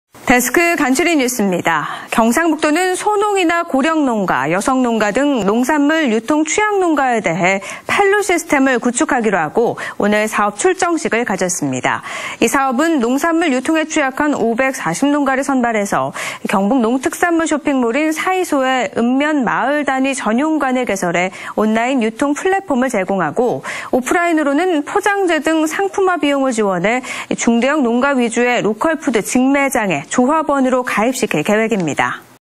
경북도, 취약농가 판로 확대 지원 출정식 / 안동MBC
2020/06/08 11:49:14    작성자 : 이정희

경상북도는 소농이나 고령농가, 여성농가 등
농산물 유통 취약농가에 대해
판로 시스템을 구축해 주기로 하고
(오늘, 8) 사업 출정식을 가졌습니다.
◀ＥＮＤ▶

이 사업은
농산물 유통에 취약한 540 농가를 선발해
경북 농특산물 쇼핑몰인 '사이소'에
읍,면,마을 단위 전용관을 개설해
온라인 유통 플랫폼을 제공하고,

오프라인으로는 포장재 등 상품화 비용을
지원해 중,대형 농가 위주의 로컬푸드 직매장에 조합원으로 가입시킬 계획입니다.